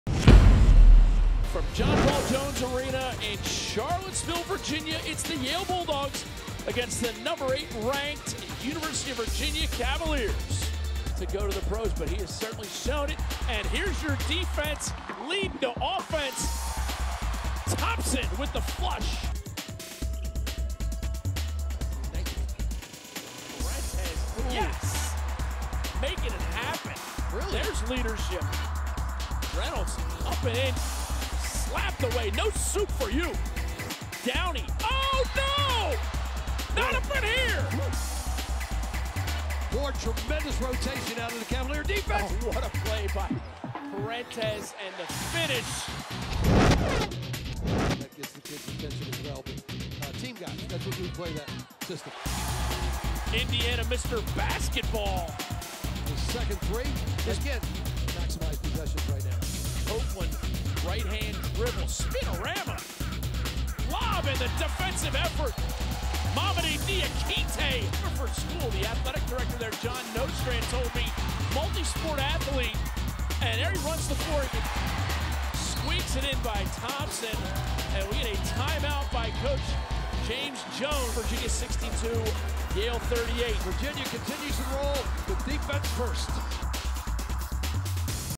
Virginia vs. Yale Men's Basketball Highlights (2016-17)
Virginia vs. Yale: The #8 Cavaliers held the Bulldogs scoreless for almost 9 minutes in the 2nd half and won going away, 62-38. Marial Shayok scored 15 points and Mamadi Diakite had several monster blocks in the victory.